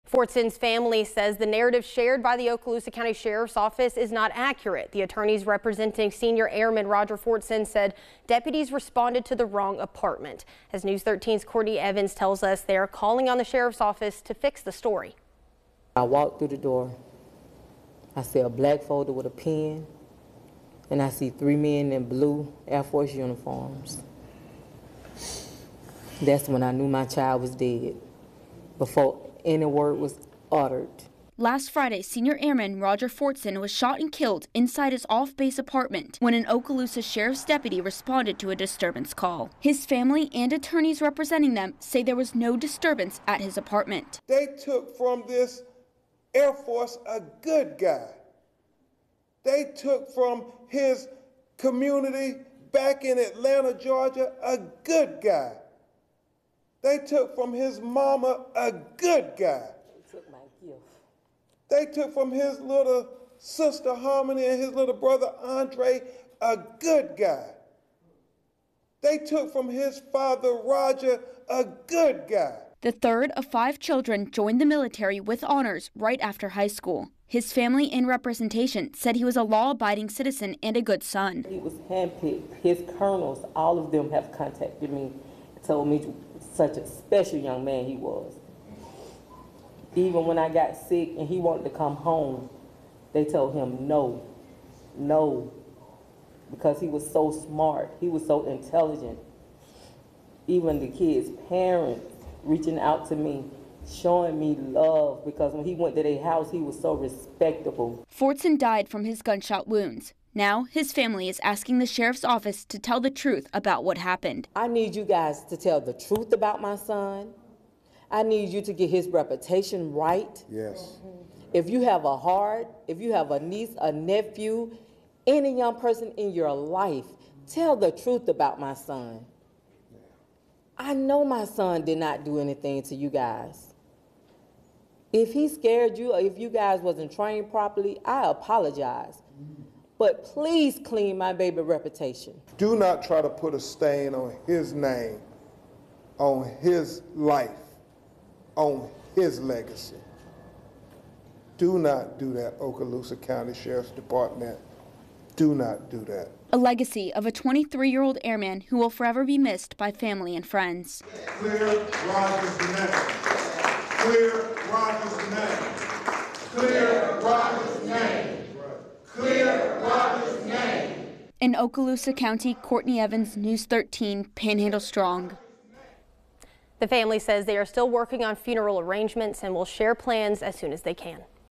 "He was a good guy", the family of an Airman killed by a deputy seeks justice
The family of an Airman who was shot and killed by an Okaloosa Sheriff's deputy said the narrative shared by the department is not accurate.